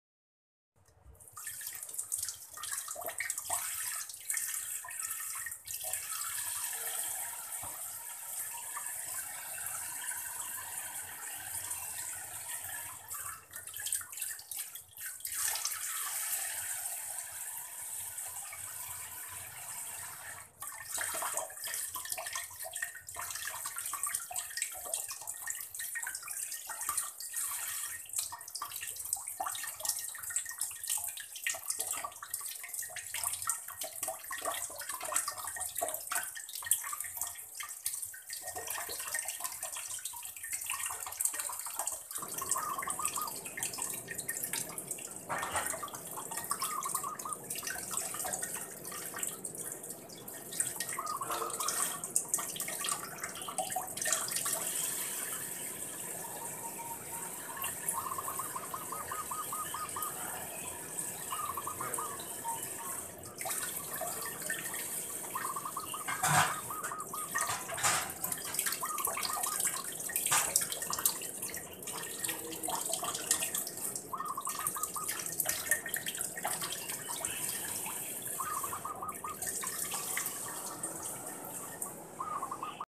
cara perkutut.Memandikan perkutut
Memandikan dan menjemur burung perkutut itu wajib dilakaukan karena suhu badan perkutut harus setabil agar performa kesehatan ,anggunganya bisa setabil